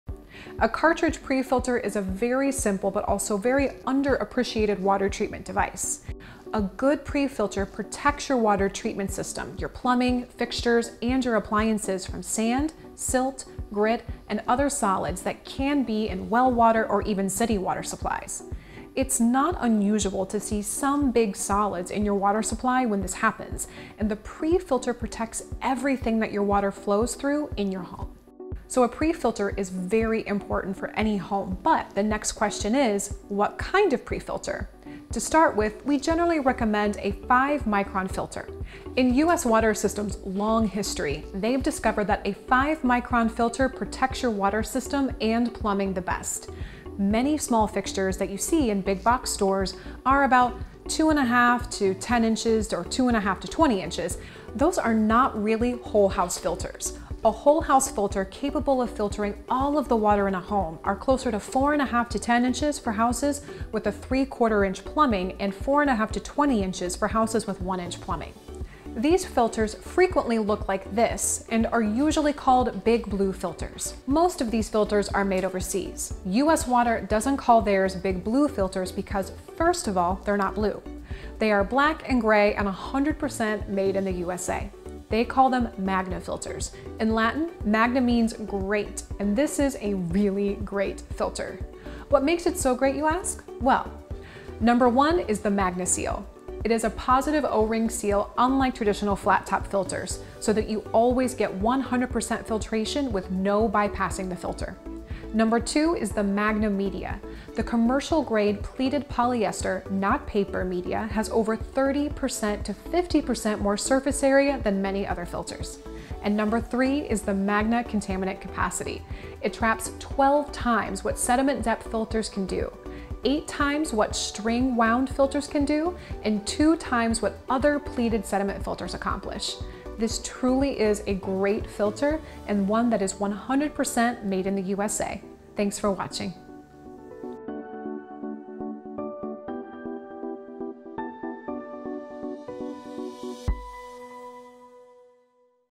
The Best Pre-Filter For Your Water Treatment System
Magna is a Latin word which means great, and that is the true and accurate meaning of the US Water Magna Filter. Learn why this US made filter, is the very best when it comes to pre-treatment and filtration.